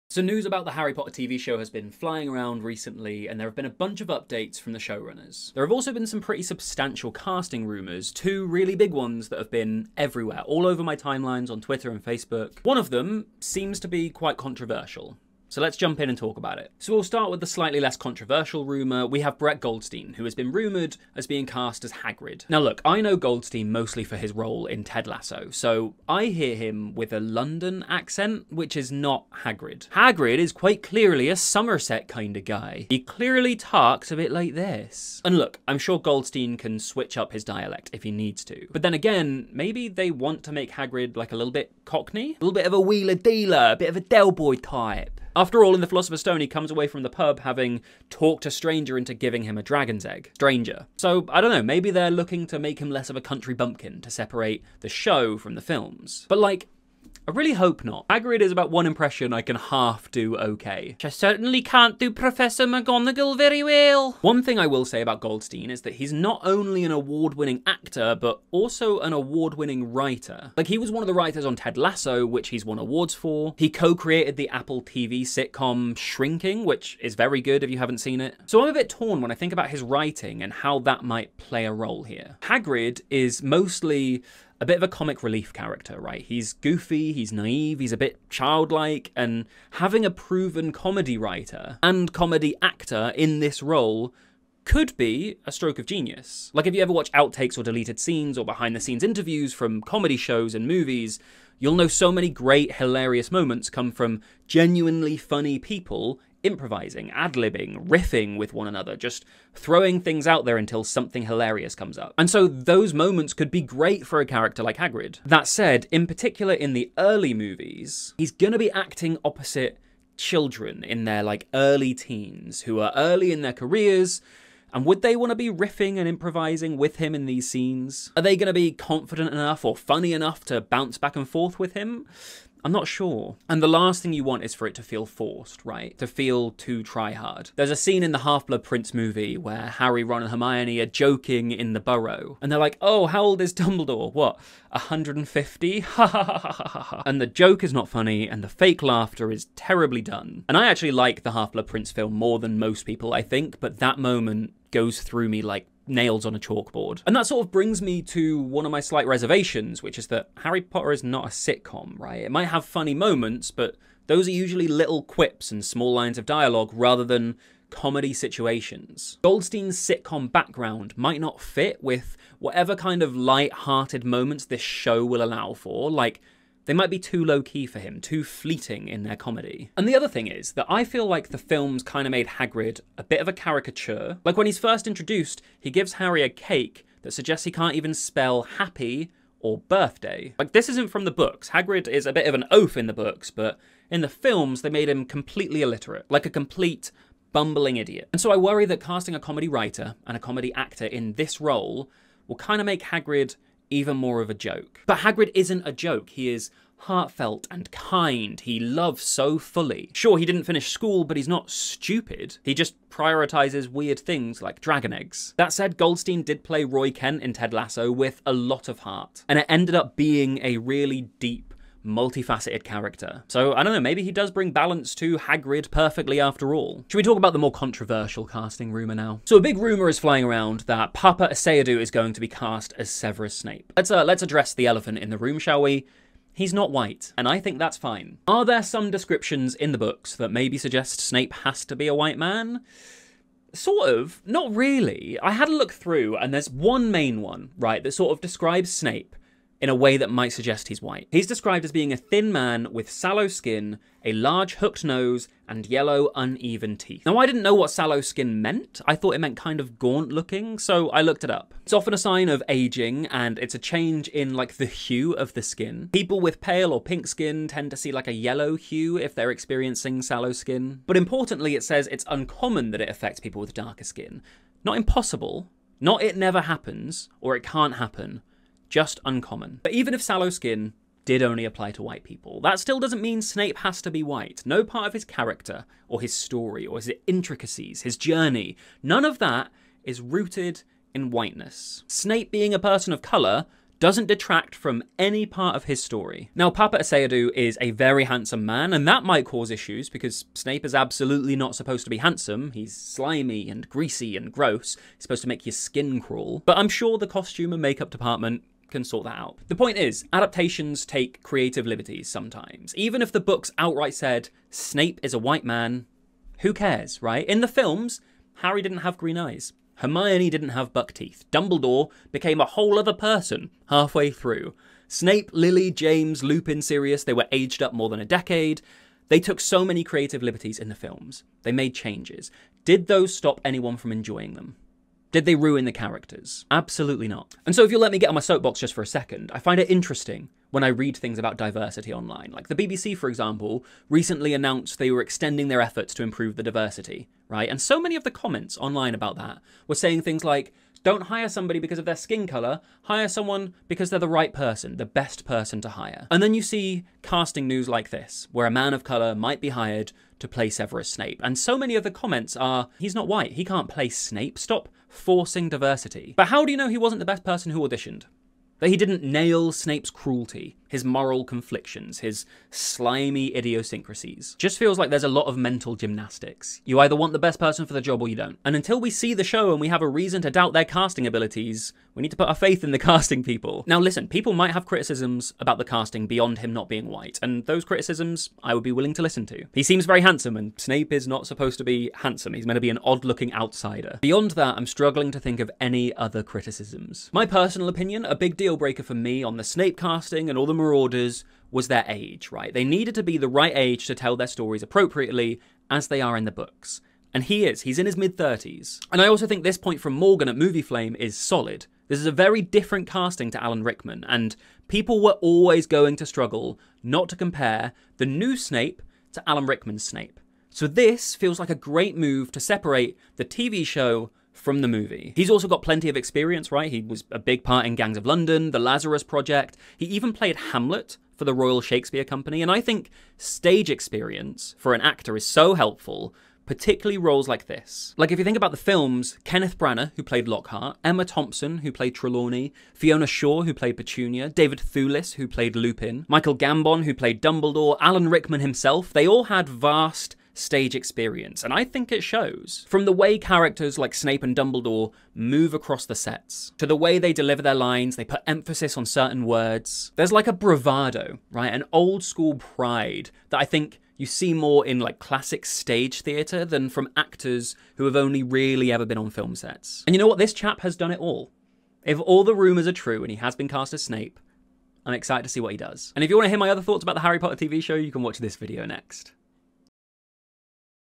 Casting News for Harry Potter TV Show?!
The rumour mill has been flying about the new Harry Potter TV show lately, specifically two big casting stories. Brett Goldstein as Hagrid, and Paapa Essiedu as Severus Snape.

Here are my thoughts about them as options for these characters, and my thoughts on the comments the rumours have caused.